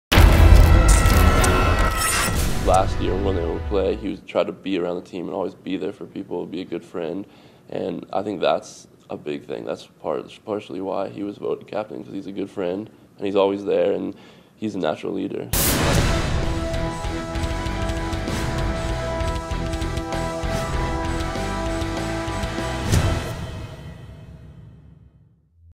Hawaii's Ben Clarke On Quarterback Max Wittek | CampusInsiders
The Rainbow Warriors' Ben Clarke spoke at the 2015 Mountain West Media Days about new quarterback transfer Max Wittek.